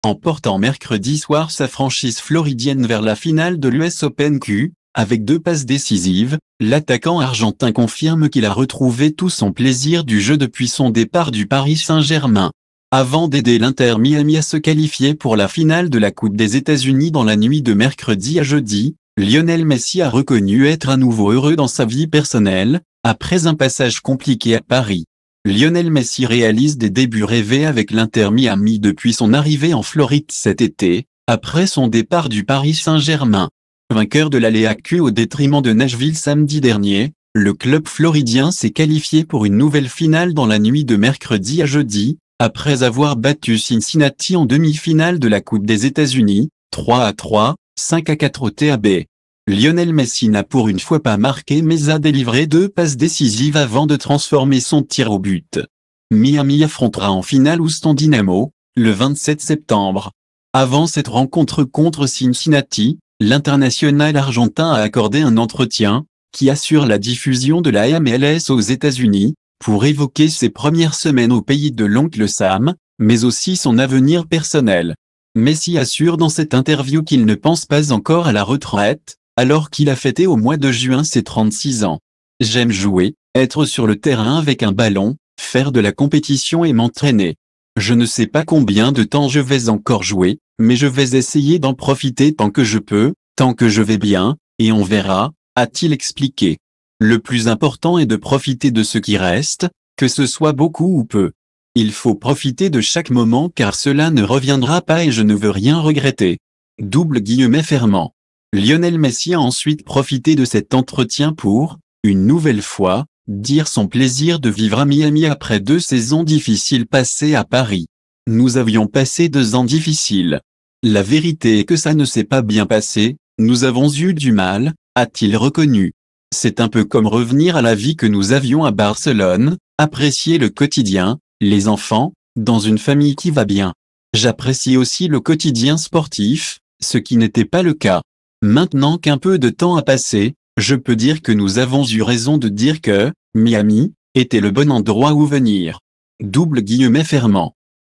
Lionel Messi parle de sa retraite
Le plus important est de profiter de ce qui reste, que ce soit beaucoup ou peu. Il faut profiter de chaque moment car cela ne reviendra pas et je ne veux rien regretter. »